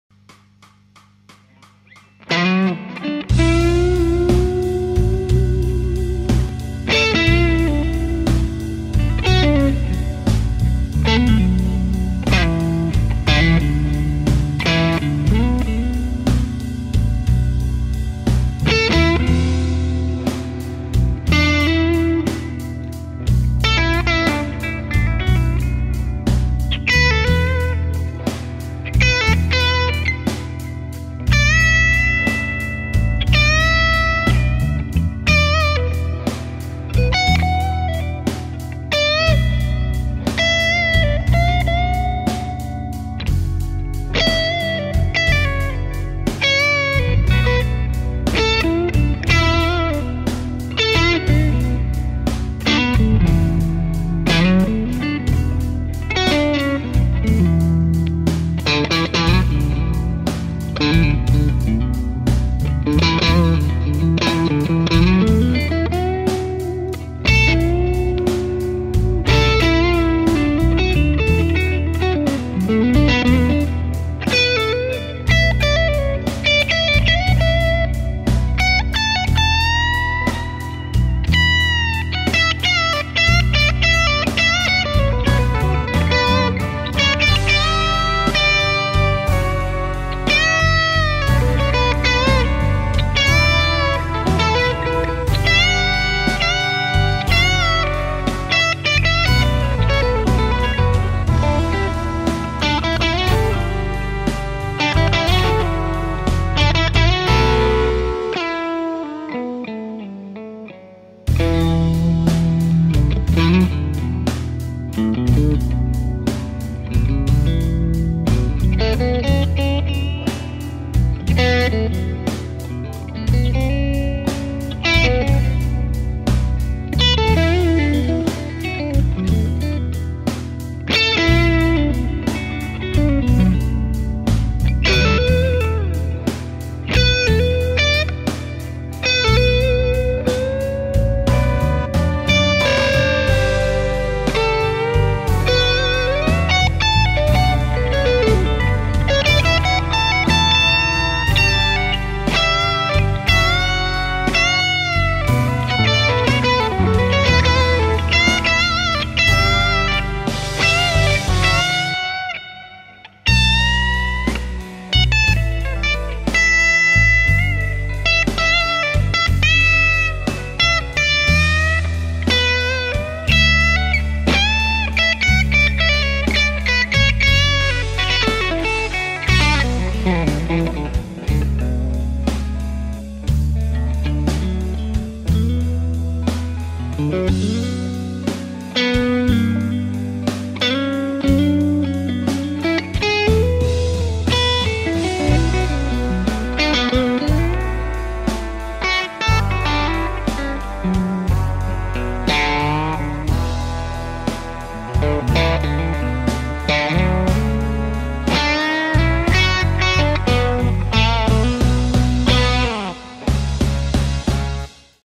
1999 Fender Telecaster Am. Std. Part2 (overdrive)
Okko Diablo Overdrive, Amp: Brunetti Single Man 35, T-Rex Reptile Delay,